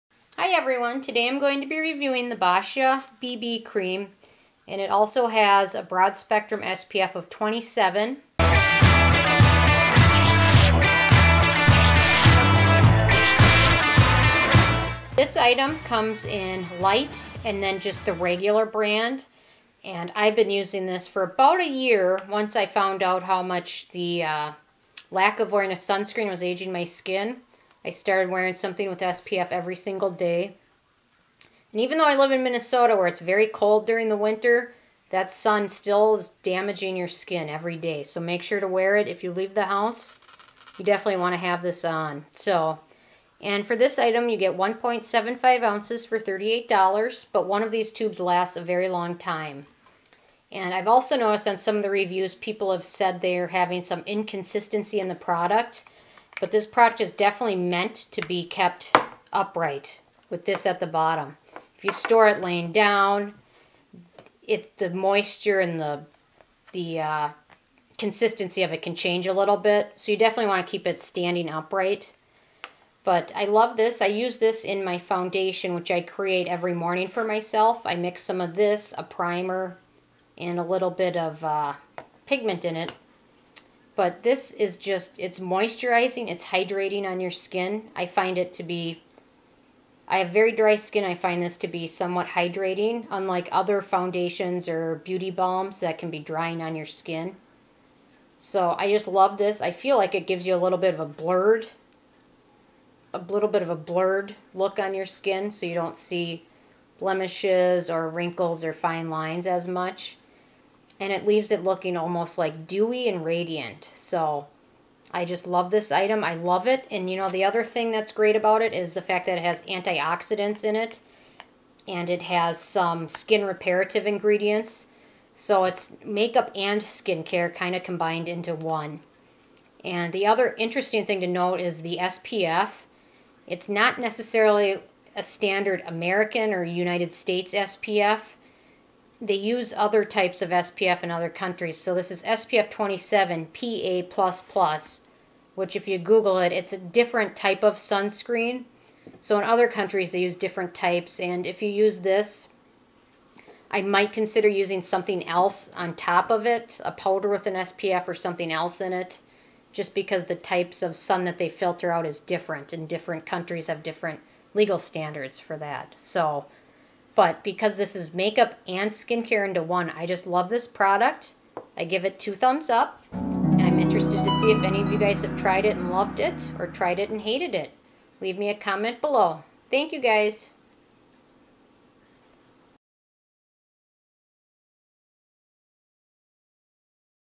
Boscia BB Cream with broad spectrum spf 27 pa++ Review and Swatch
I use the Boscia BB Cream with broad spectrum spf 27 pa++ many days a week an integral part of my makeup routine.  This has a much more natural looking finish and I love that this product has a lot of beneficial ingredients, such as the skin lightening antioxidant Arbutin.  This product leaves a nice glowing finish and I love that it contains some sunscreen- I love using as many products with it as I can.